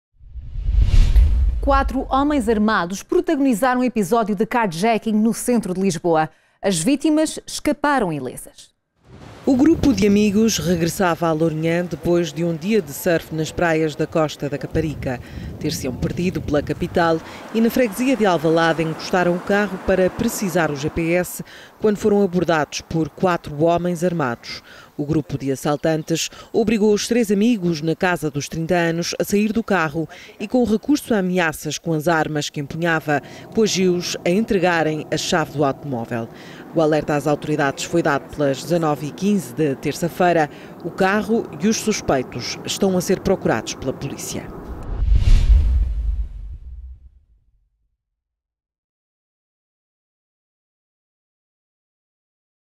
Carjacking em Lisboa
Quatro homens armados protagonizaram um episódio de carjacking no centro de Lisboa. As vítimas escaparam ilesas.